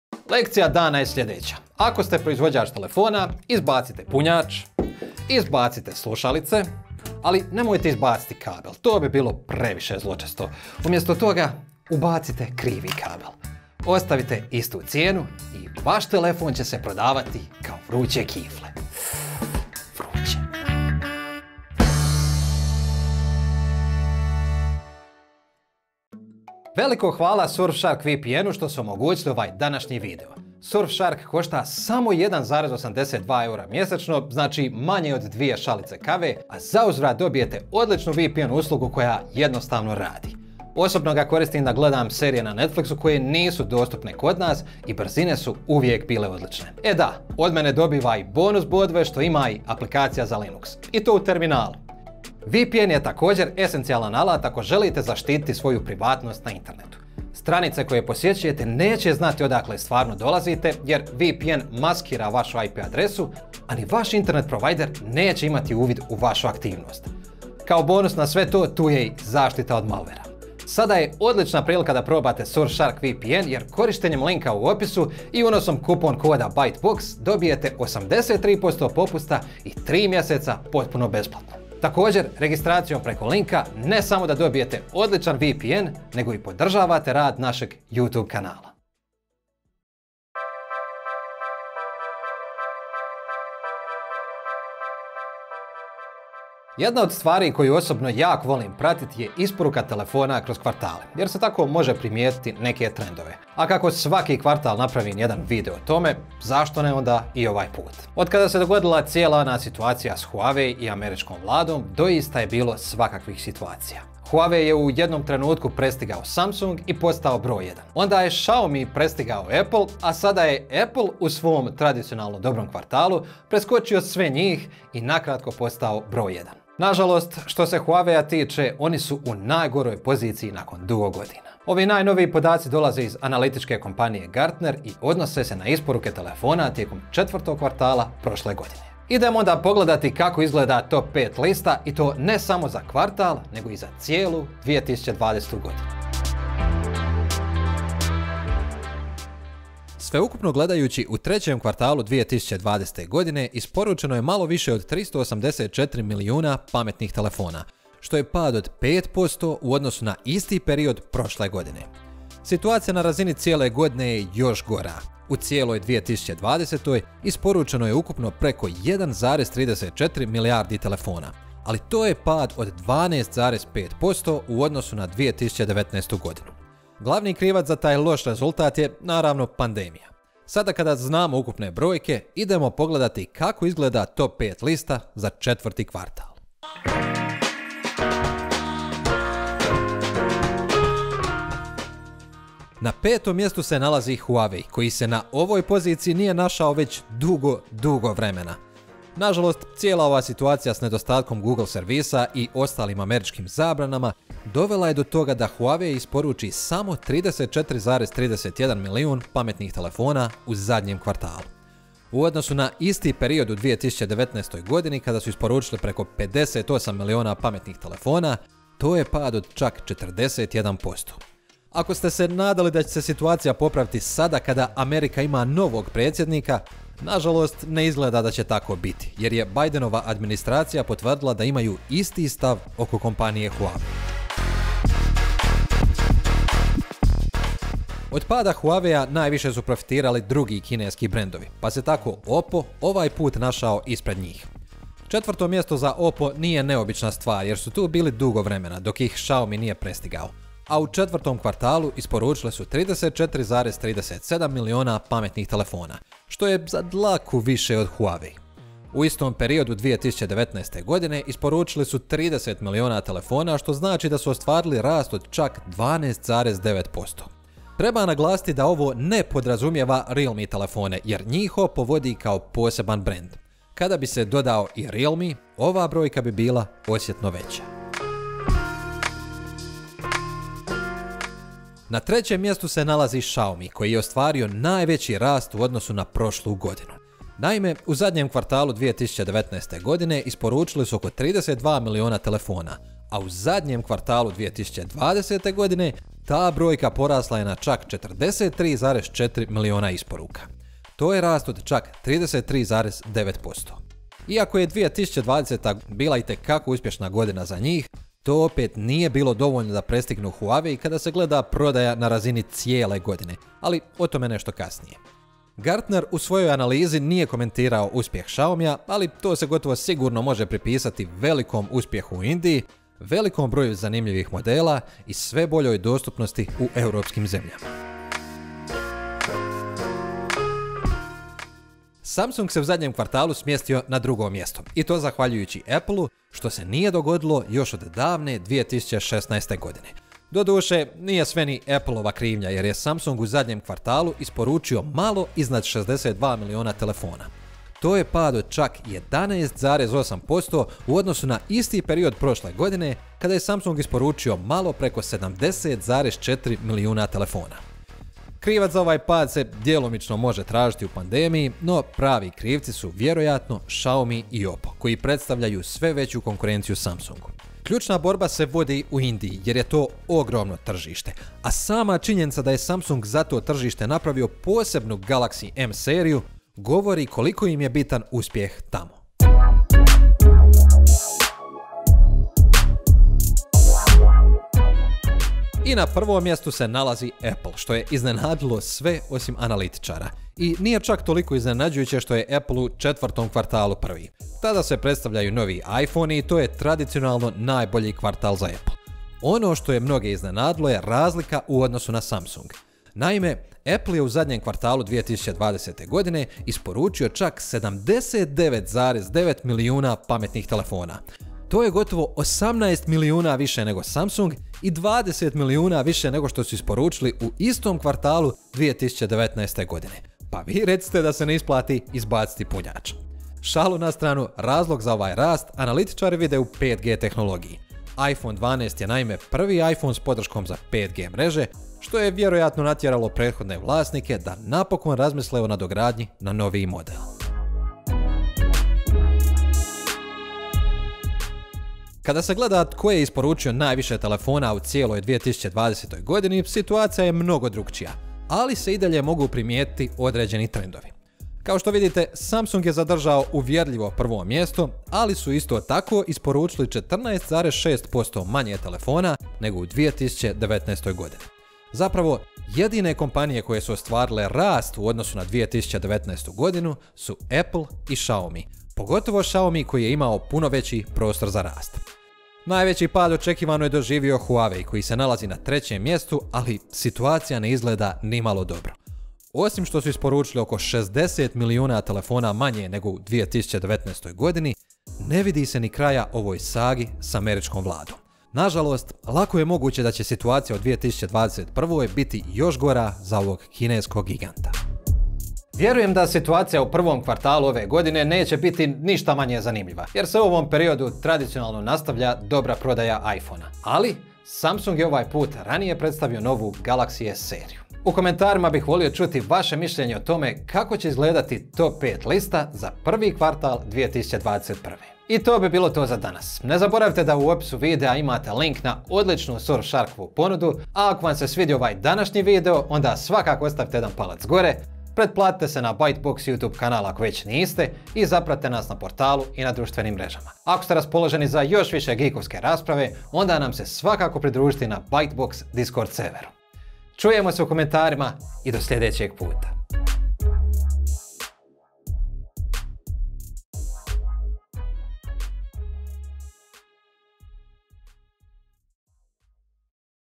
Apple je PRESTIGAO Samsung (2020 Q4)! Prodaja telefona u 2020. godini...
Posjeti unesi promo kod BAJTBOX za 83% popusta i 3 dodatna mjeseca koje dobivaš besplatno! Video sponzorira Surfshark VPN.

Apple je očekivano prestigao Samsung u zadnjem kvartalu 2020. Ali malo tko se nadao ovakvoj razlici...